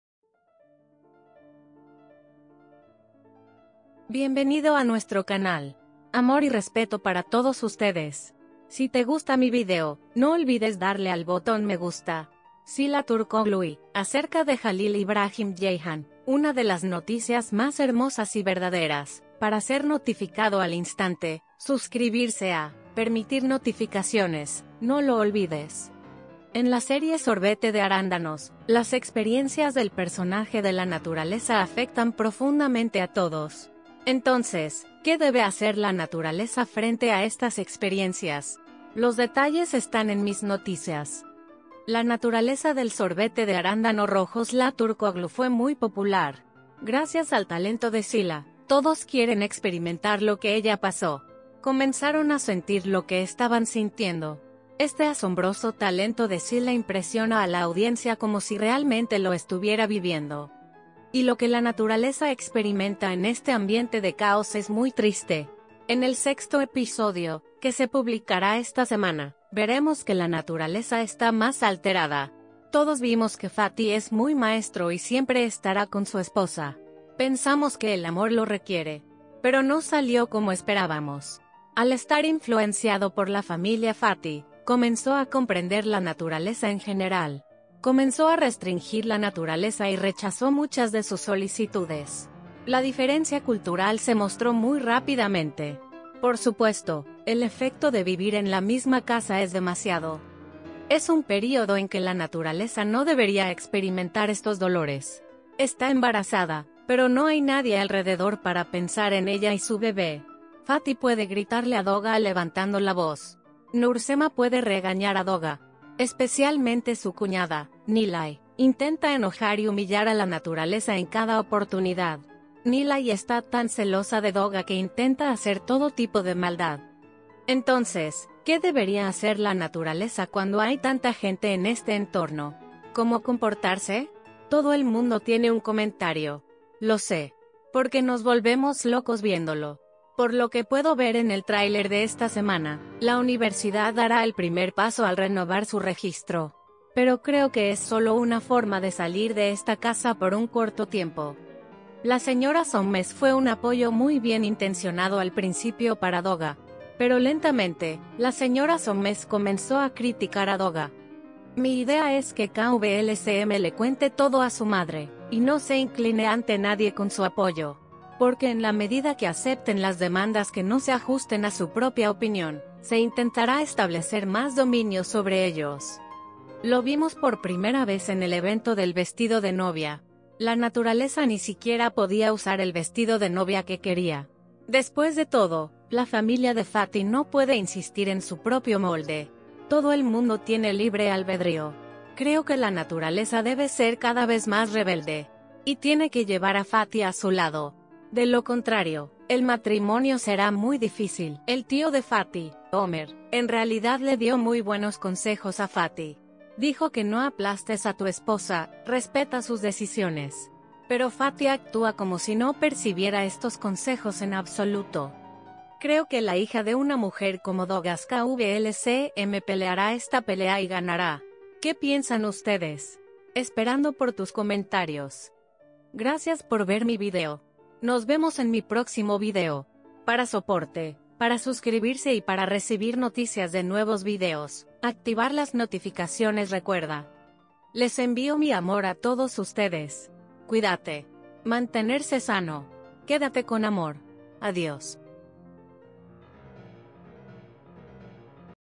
¿Qué debe hacer la Doğa?
En la serie Sorbete de arándanos,
Las experiencias del personaje de la naturaleza afectan profundamente a todos.
Entonces, ¿qué debe hacer la Naturaleza frente a estas experiencias?
Los detalles están en mis noticias..